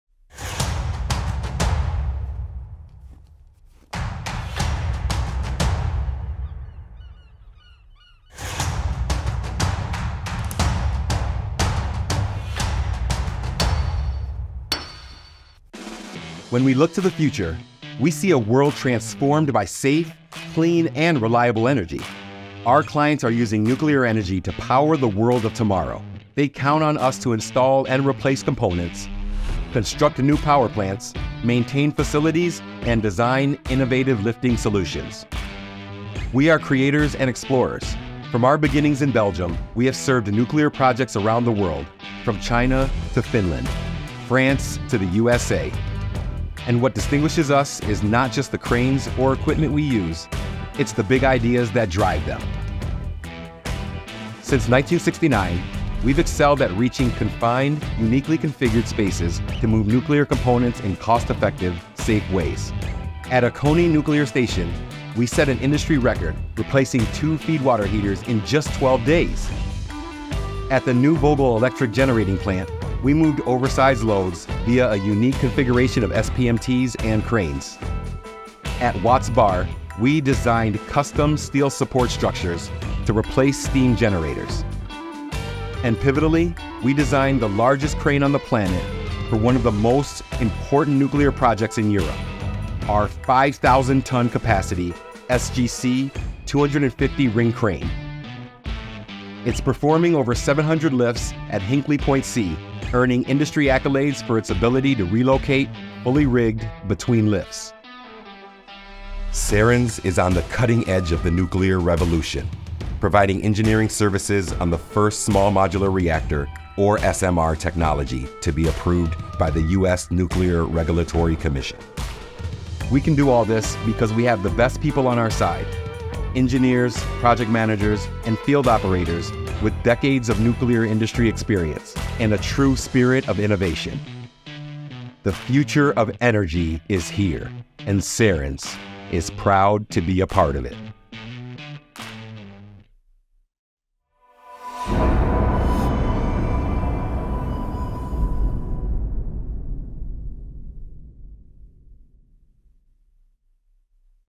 Sarens Creators & Explorers – Our Nuclear Expertise is Creating a Cleaner Future
When we look to the future, we see a world transformed by safe, clean, and reliable energy.

At Sarens, that vision drives us to work with forward-thinking clients who are harnessing nuclear energy to power the world of tomorrow. They count on us to install and replace critical components, construct new power plants, conduct long-term maintenance, and develop faster, safer, and smarter solutions for doing so.